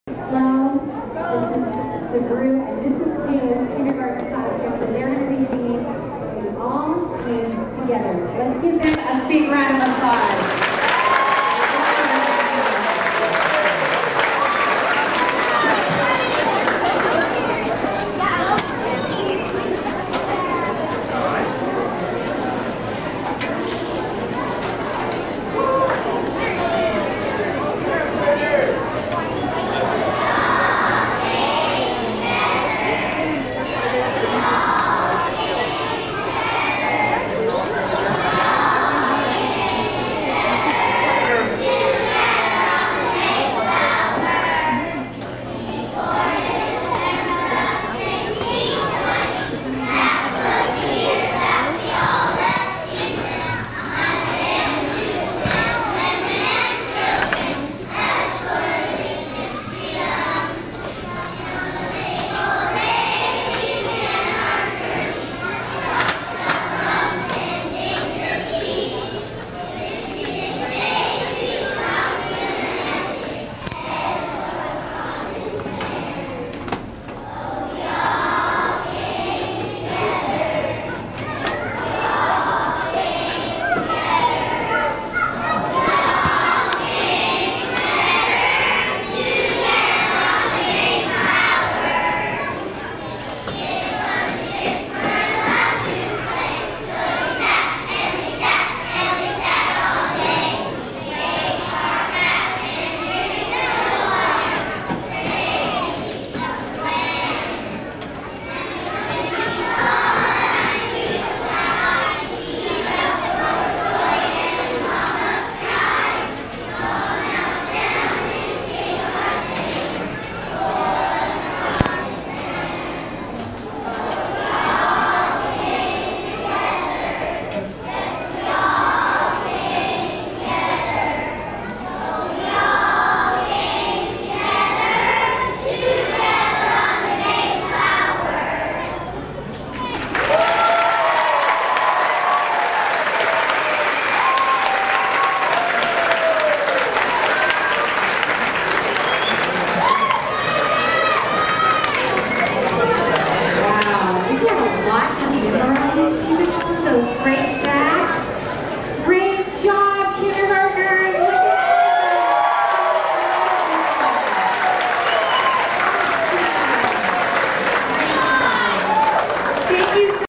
Zachs school play
Endeavour Elementary Mayflower Play. Kindergartners singing